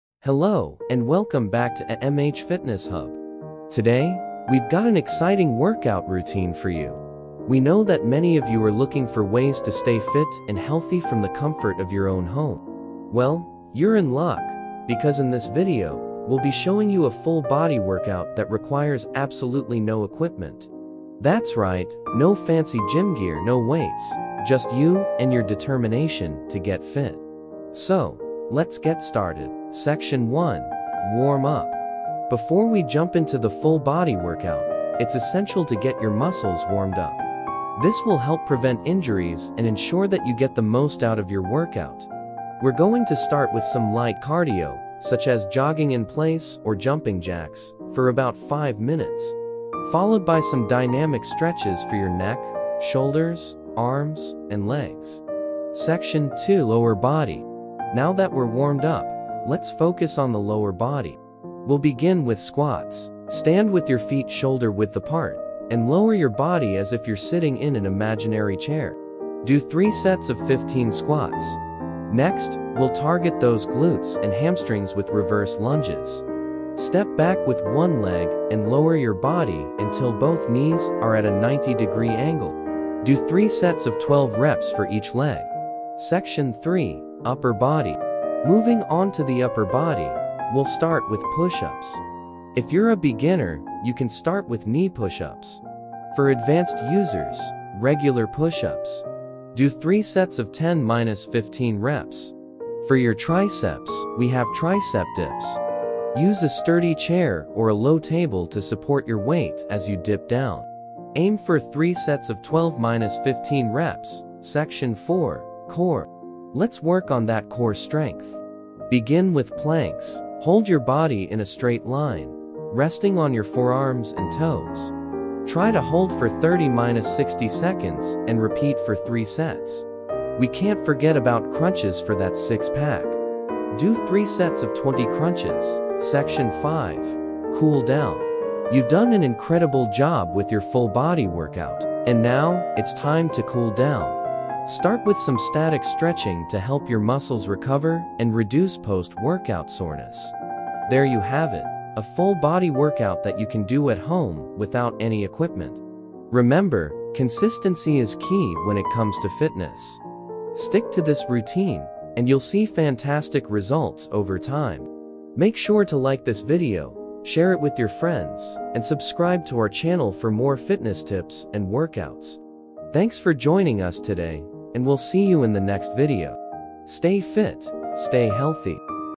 Full Body Workout at Home | No Equipment | 7-Day Home Workout Plan | Burn Fat & Gain Muscles
Welcome to MH Fitness Hub! Get ready for a complete full body workout without the need for any gym equipment. In this video, we'll guide you through a comprehensive seven-day workout plan, targeting every muscle group - biceps, back, triceps, legs, chest, and shoulders, all from the comfort of your home. Whether you're a beginner or an experienced fitness enthusiast, this routine is perfect for everyone.

🏋️‍♀️ Discover the best full body workout routines that require no dumbbells or special equipment, making it accessible for anyone.

💪 Looking to gain lean muscle or burn fat? You're in the right place! We'll show you effective exercises to help you achieve your fitness goals.

🥗 Plus, we'll provide insight into the best protein sources and a full day of eating to support your muscle gains and overall fitness.

🔥 Don't forget to incorporate our abs workout and cardio exercises at home for optimal fat loss. Achieve the body and fitness level you've always wanted!

Join us on this fitness journey and boost your body's strength and stamina. It's time to unlock your potential and become the best version of yourself. Let's get started! 💪🏡🏃‍♀️